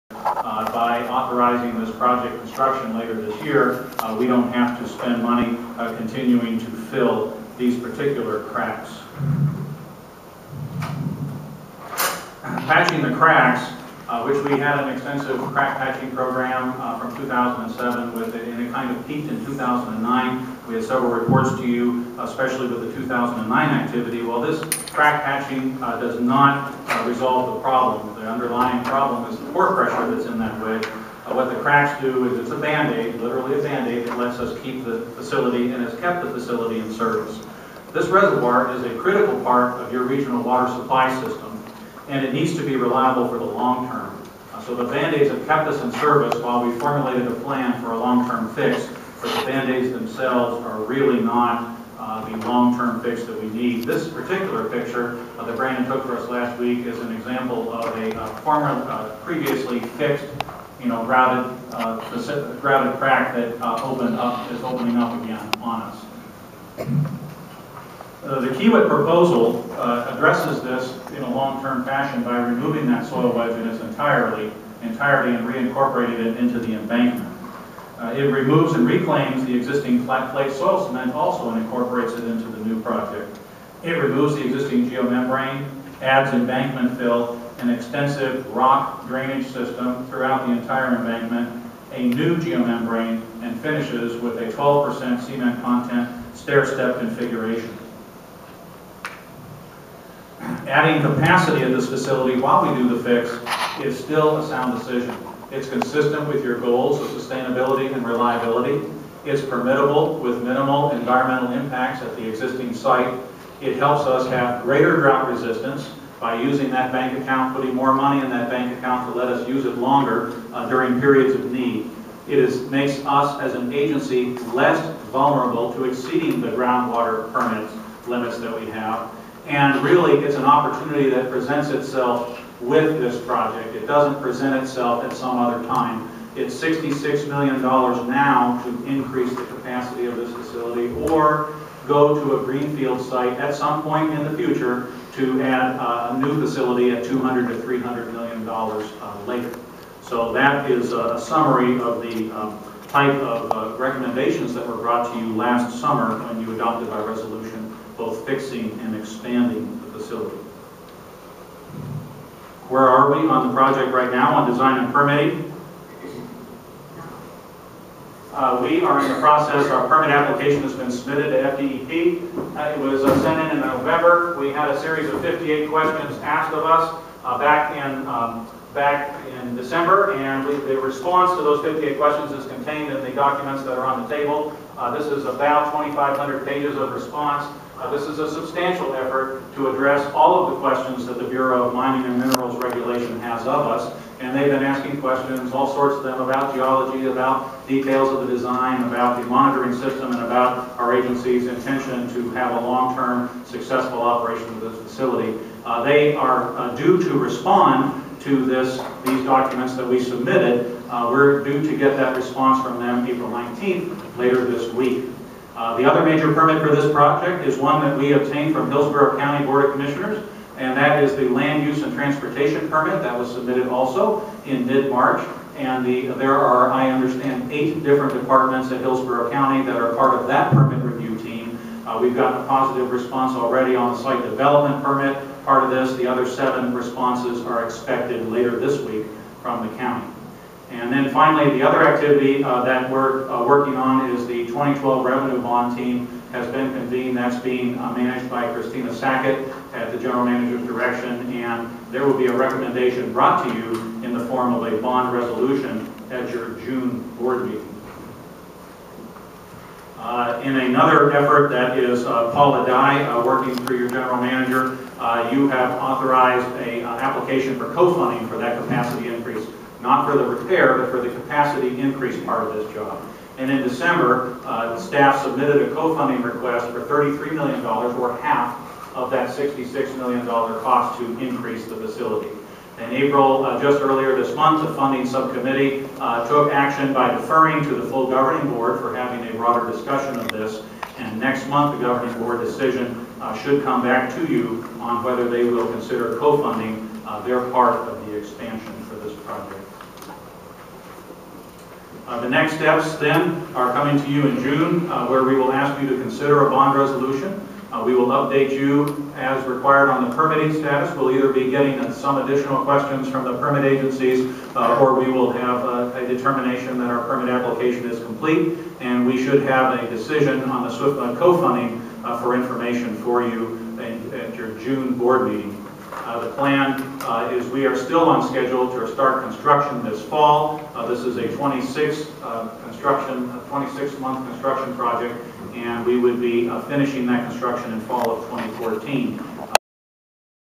Florida's largest reservoir awaits repairs to growing cracks after lawsuit defeat: WMNF News
Tampa Bay Water's Jon Kennedy, project manager for the reservoir's repairs, explains what needs to be done and how long it will take.  Completion is expected in The fall of 2014 with construction to begin this fall.